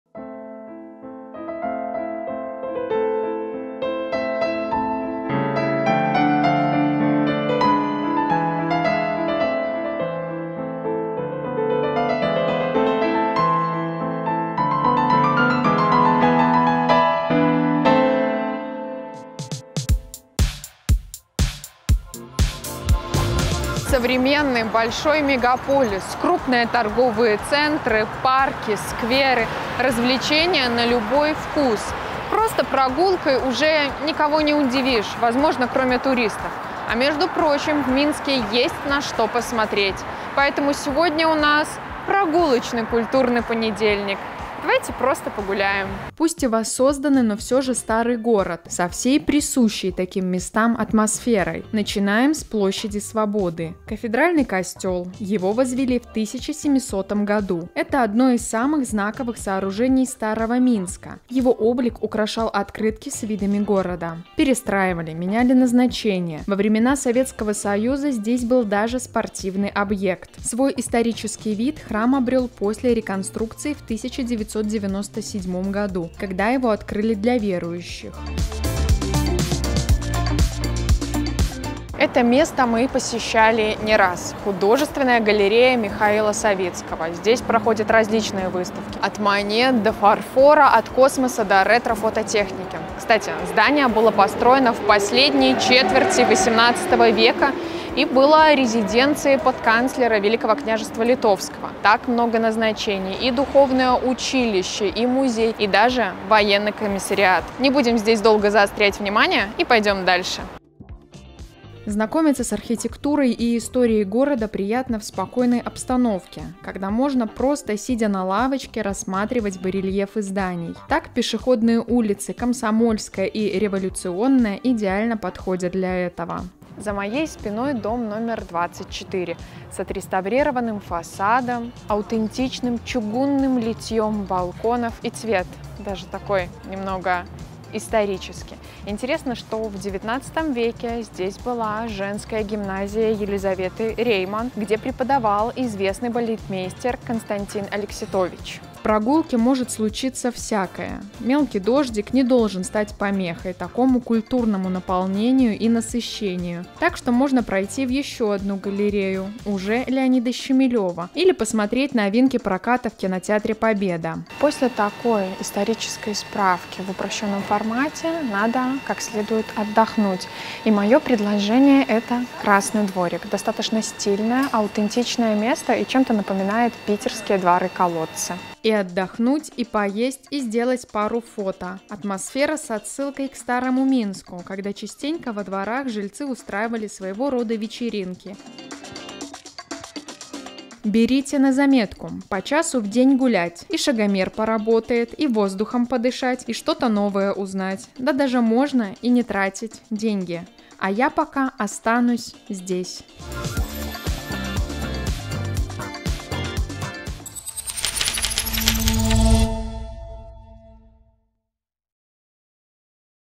Есть, что посмотреть II Пешком по улицам столицы II Прогулочный культурный понедельник
Минск - современный большой мегаполис, крупные торговые центры, множество заведений с самой разнообразной кухней, парки, скверы. К этому мы уже привыкли, просто прогулкой уже мало кого удивить. А, между прочим, посмотреть в столице, определенно, есть на что. Интересный и исторический маршрут в нашем сюжете.

Над сюжетом работали: Валентина Трубач, Илона Глинистая.